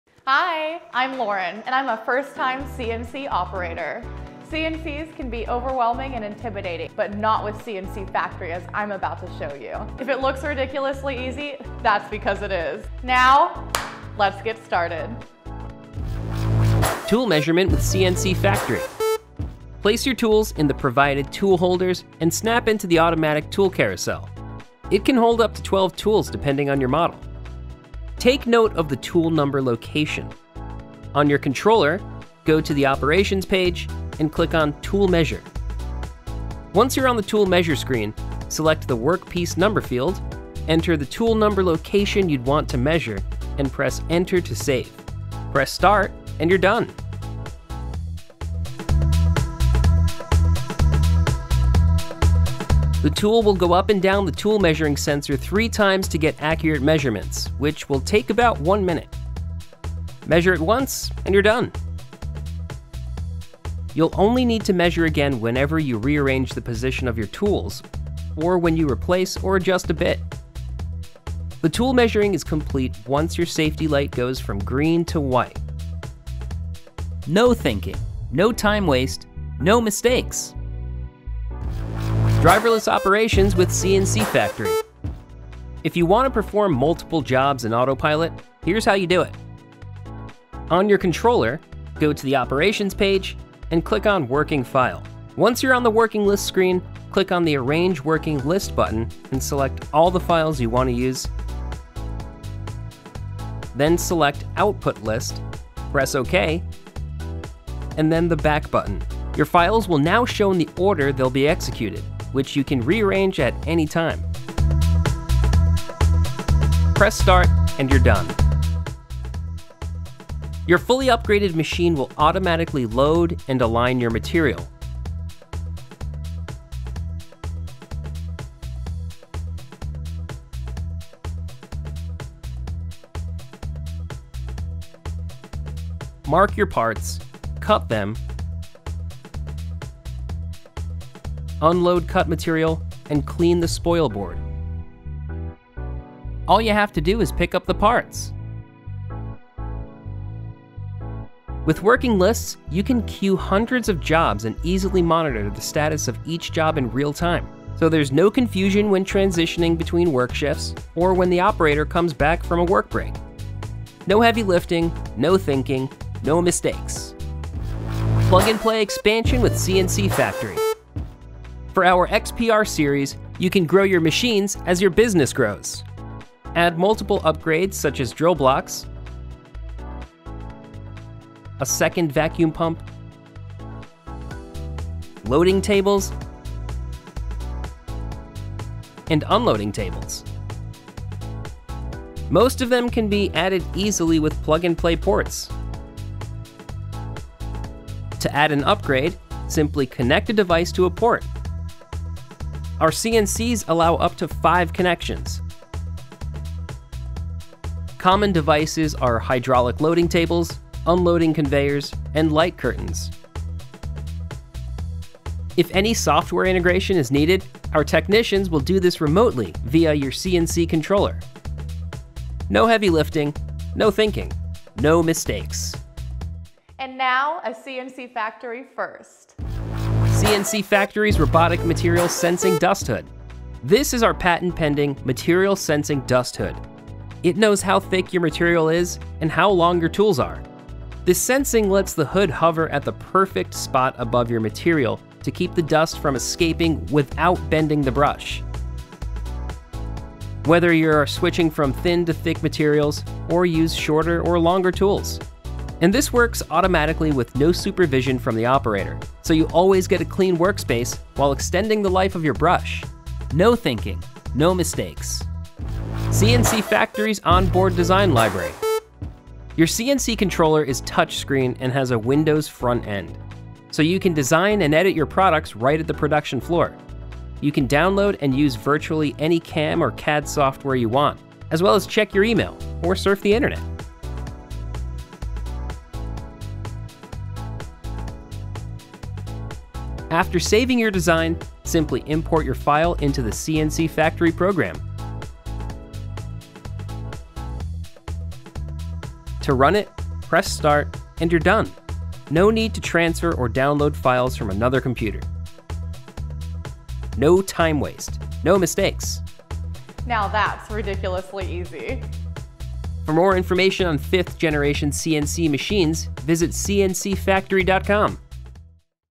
Ridiculously Easy CNC Operations with CNC Factory
First-time CNC Operator, Loren, shows you how ridiculously easy it is to operate 5th Generation CNC Routers. No experience or G-code knowledge needed, and it only takes 1 operator! From measuring tools to running everything in autopilot, CNC Factory makes automation easy at a very accessible price. Financing offered.

00:16 Tool Measurement
01:22 Driverless Operations
02:48 Plug & Play Expansion
03:46 Robotic Material Sensing Dust Hood
04:26 Onboard Design Library

Find us anywhere with TheCNCFactoryExperience